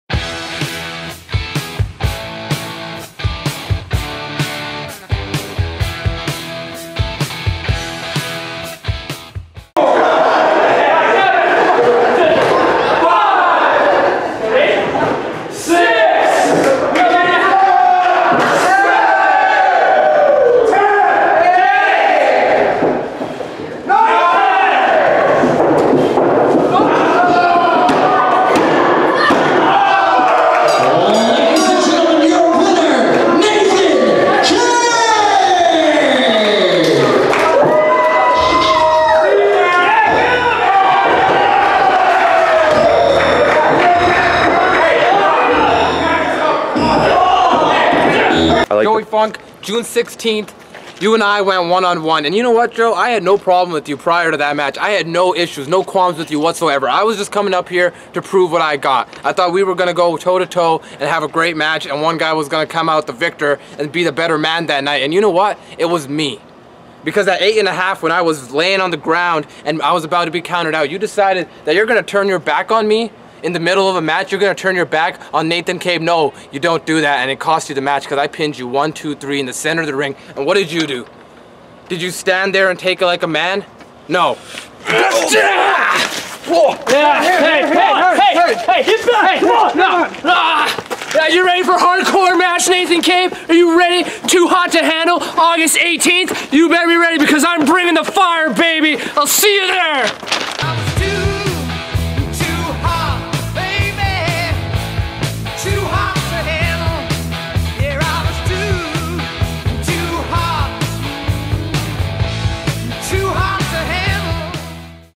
The Kabe/Funk Rivalry Explodes!
Nathan Kabe didn't realize how unhinged Joe Funk could become after beating him in June: threats of violence and match interference has escalated this situation beyond control, it seems. The only way to settle this, is with a good ol' fashioned WEAPONS MATCH!

Big West Wrestling presents 2 HOT 2 HANDLE on Friday, August 18 at Lake City Bowling in Kelowna. Doors at 7:30pm. All ages event, with a licensed area for the adults to enjoy a cold one while watching some red-hot wrestling action. DONT'CHA DARE MISS IT!